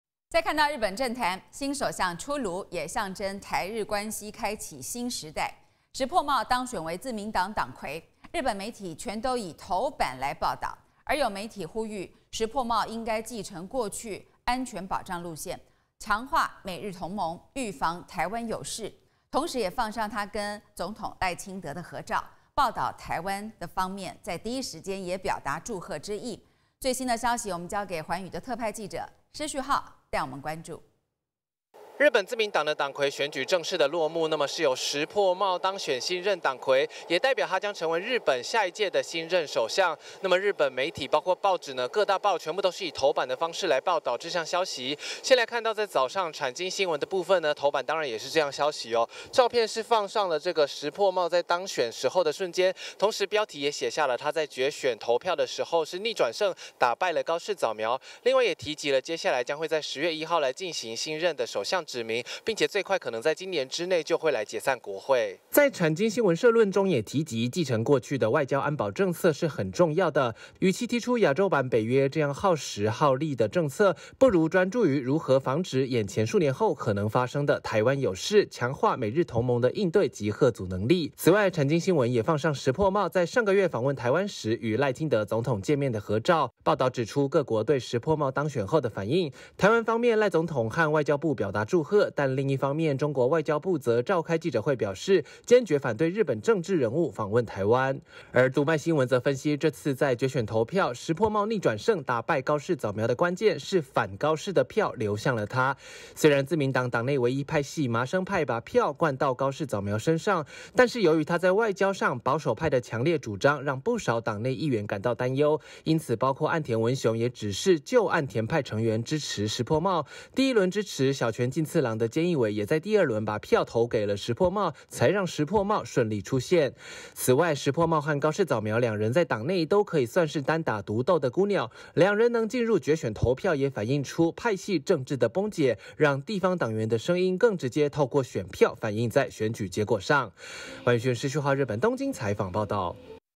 石破茂將成新日相！「反高市」岸田票相挺成勝出關鍵 ｜寰宇新聞
石破茂當選日本自民黨黨魁，他也將成為日本新任首相。日本媒體全都以頭版來報導這項消息，當中有媒體呼籲石破茂應繼承過去的安全保障路線，強化美日同盟、預防台灣有事，同時也放上他和賴清德的合照，報導台灣方面在第一時間表達祝賀。